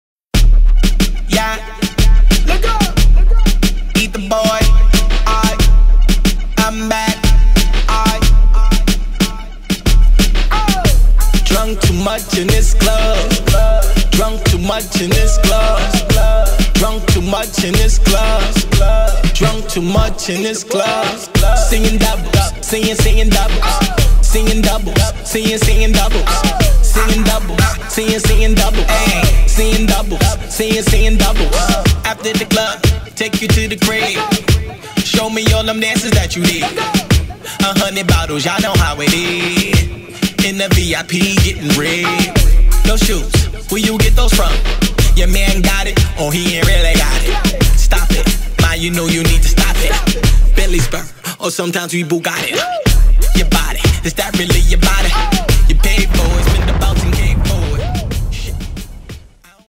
urban fridays club cielo bahrain.mov
urban fridays at club cielo bahrain with dj riddler and dj westside